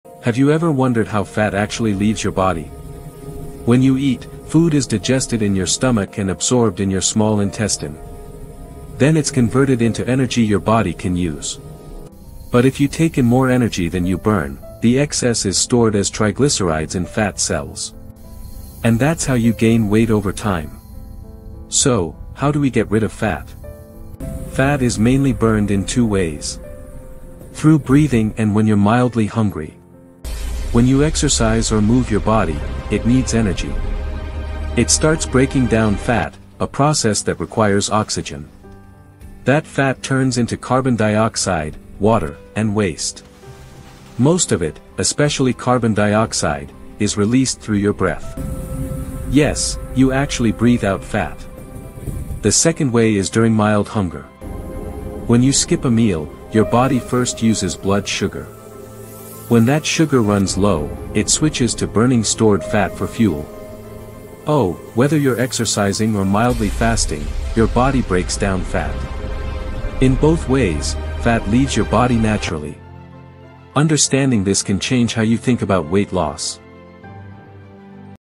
“You Actually Breathe Out FAT! 😳
Most people think fat leaves the body through sweat… but that’s not true!
Here’s the REAL science:

✔️ Fat is stored as triglycerides
✔️ When your body burns fat, it breaks into CO₂ + H₂O
✔️ 84% of fat leaves through your breath as carbon dioxide
✔️ The rest becomes water and exits naturally
✔️ Exercise + light fasting = maximum fat burning

This 30–second explanation will completely change how you think about weight loss.
Perfect for students, fitness lovers, and anyone trying to lose weight smartly!